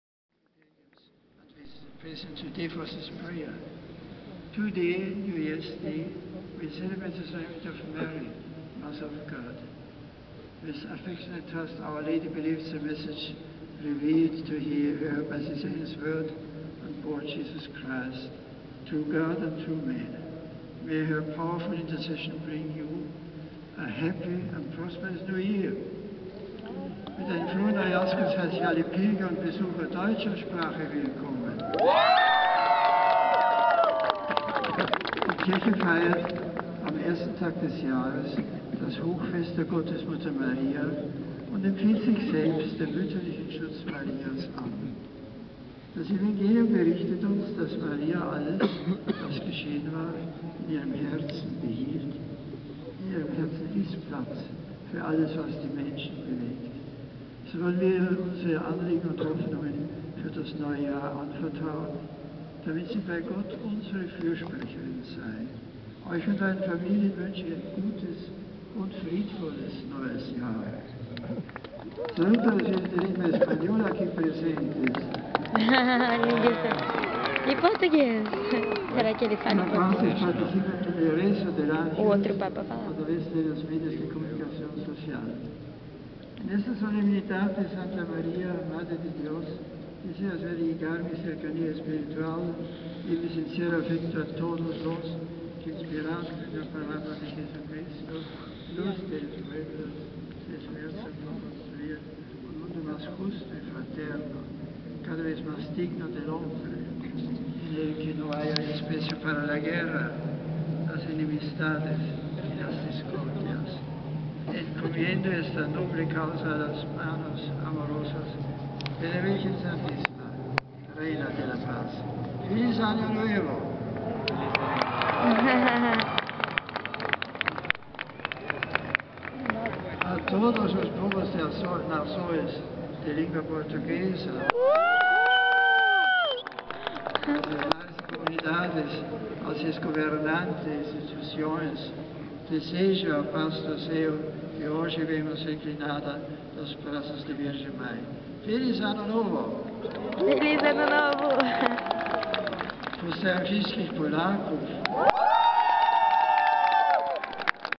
A benção do PAPA BENTO XVI - 01/01/2013
Um dos melhores dias da minha vida! Comecei o ano muito abençoada.